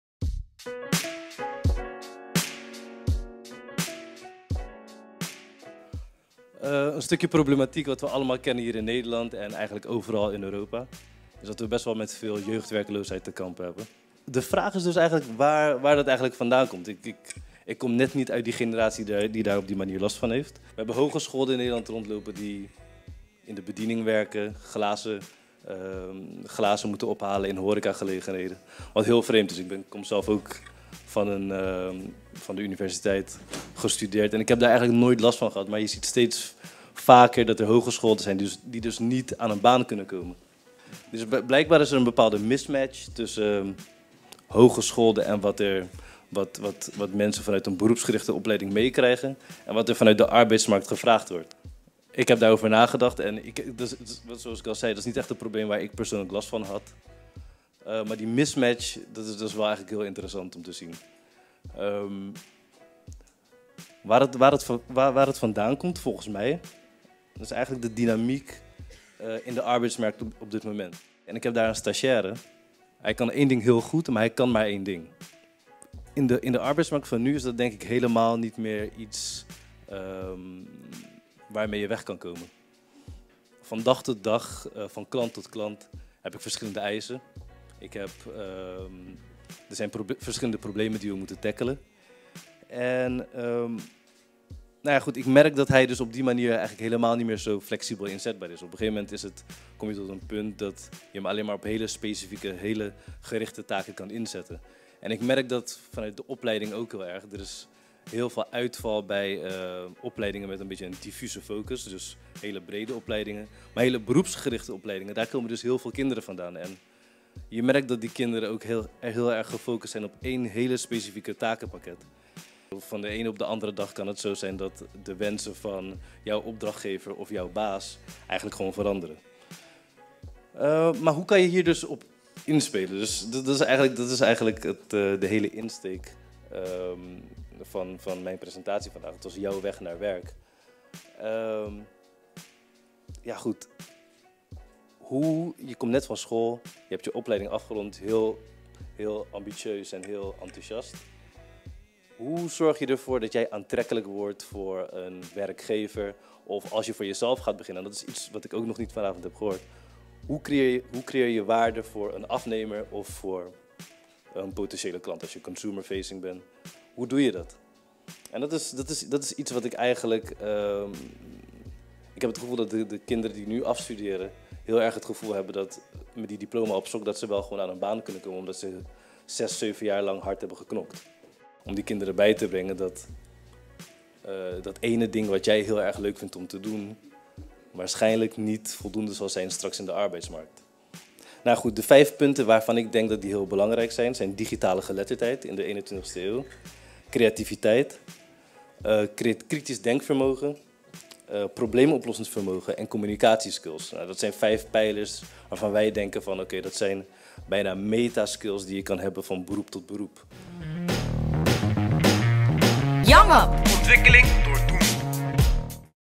Cye Wong-Loi-Sing | Jouw Weg Naar Werk
Op woensdagavond 6 mei organiseerde we de Kick-Off bijeenkomst van het "Jouw Weg Naar Werk" project. Tijdens deze avond deelde verschillende young professionals hun verhaal en hun ervaringen op de arbeidsmarkt.

In deze video vind je een samenvatting van de speech van Cye Wong-Loi-Sing. Ben je daarnaast op zoek naar meer informatie over het project?

Film- & Montagewerk door Wilmer Kousemaker